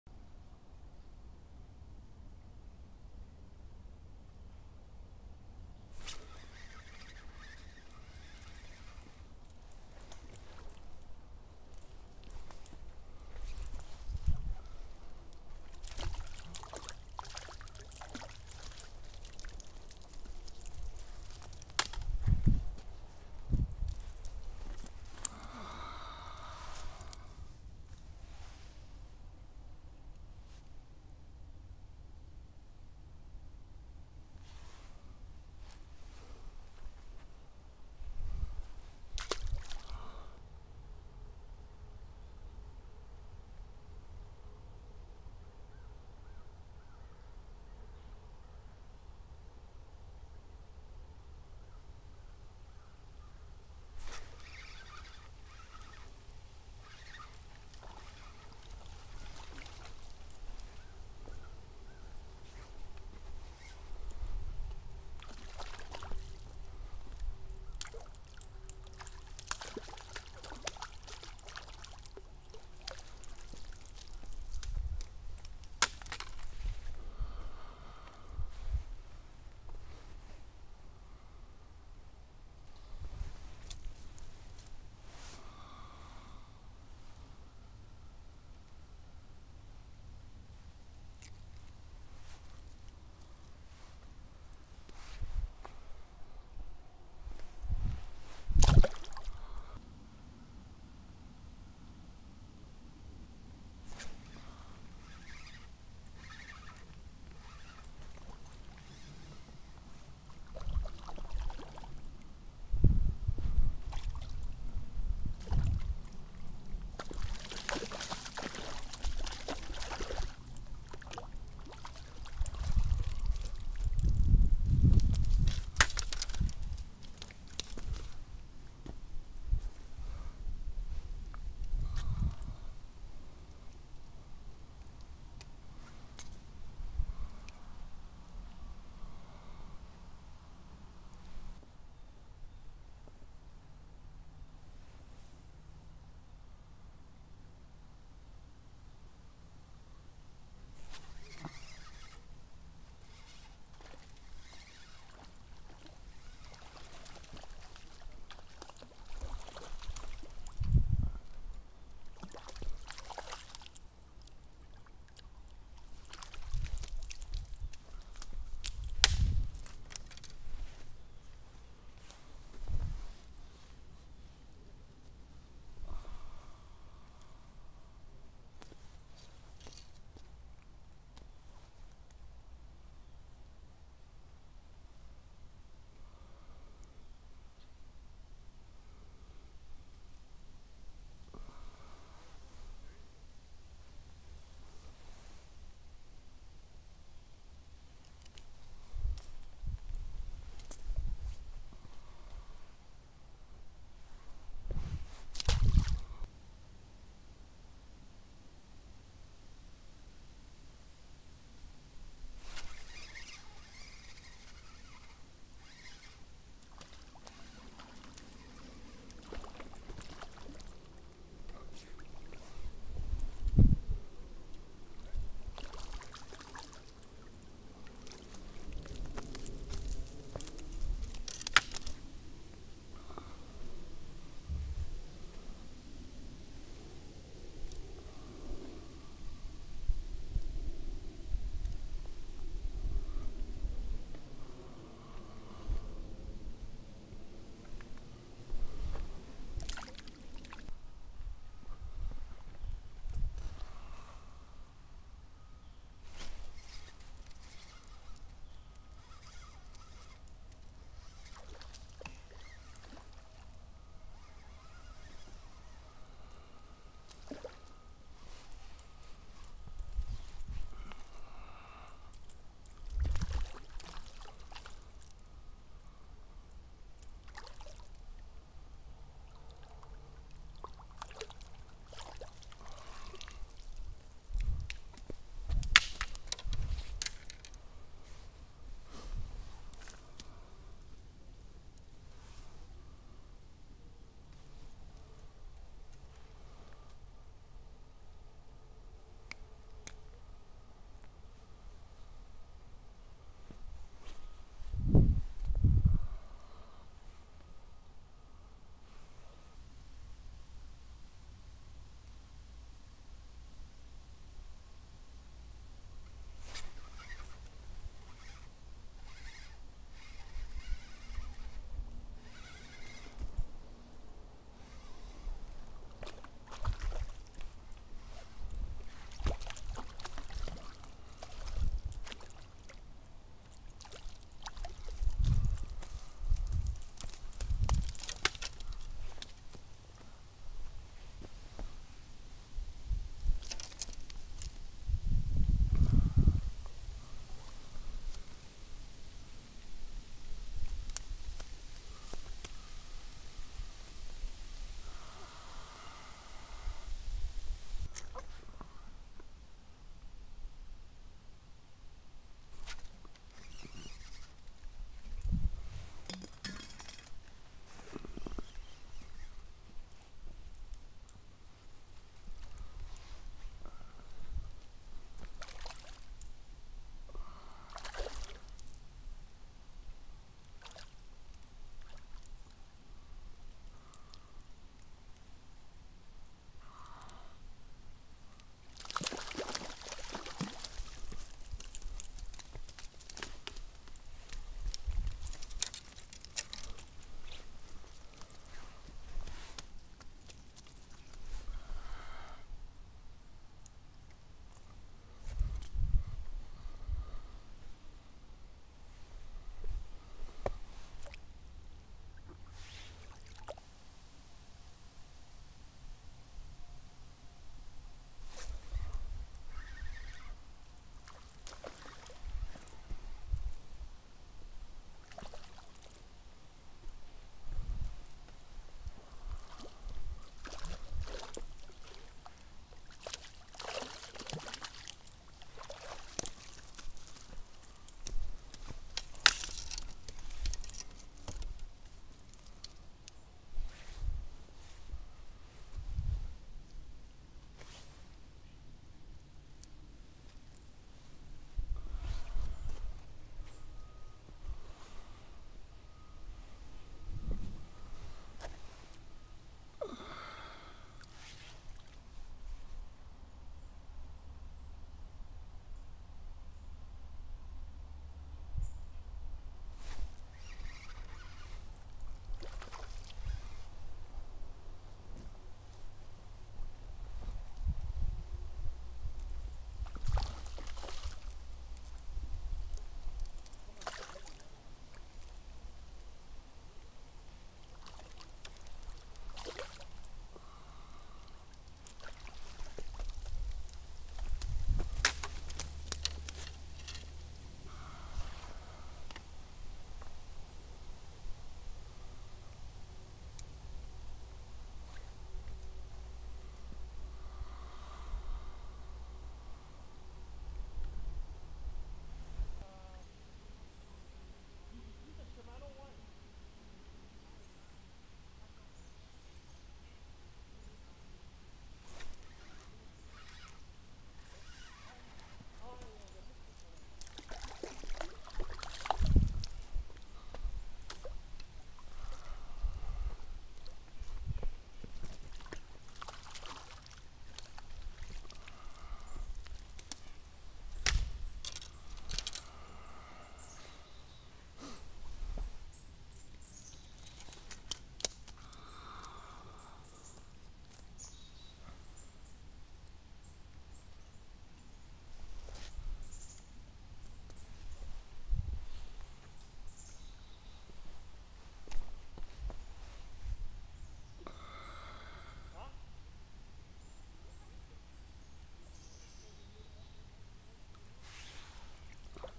October Rainbow Trout fishing at Glen Haffy - POV fishing
Date: October 3, 2018
Temperature: 16°C
Water depth: 2-4ft
Location: Glen Haffy Conservation Area, Ontario
Bait used: chartreuse trout worm (on Amazon Canada or Amazon US salmon eggs

After the trout season closes at most locations on September 30, Glen Haffy Conservation Area remains open and it has extended trout season until Thanksgiving long weekend.

The weather was sketchy, cloudy with light rain passing through but the bites were good. As usual here, I started off fishing using chartreuse worm and when the bites slowed down I switched to salmon eggs and the fun continued.

My poor float endured such a punishment over the last 2 years of setting hook and catching hundreds of fish that the brass swivel that the line goes through wore out and broke off.

When the rain started pouring, I packed up and left. As I was loading my car with the gear in a hurry, I put my rod on the roof of my car and forgot to put it inside the car. As I drove off the rod fell off and I (temporarily) lost it.